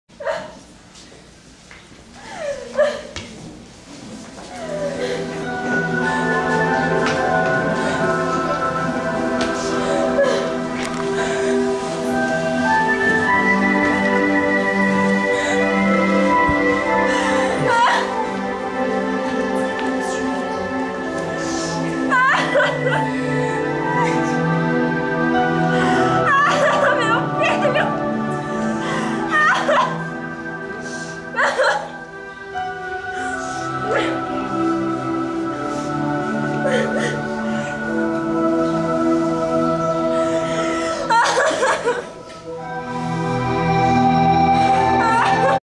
Teatro da condenação e morte de Jesus - Jocom CNSA
teatro sob minha direção apresentado para a comunidade do Lar Padre Euclides RP, Capela Nossa Senhora Aparecida, no Domingo de Ramos encenado brilhantemente pelo Grupo de Jovens JOCOM.